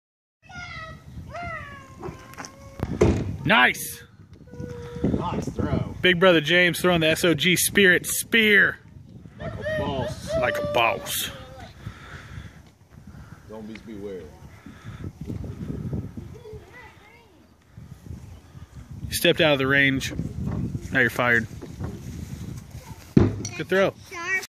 Throwing the sog speer
My big brother throwing the sog spirit speer I have on a hard wood handle.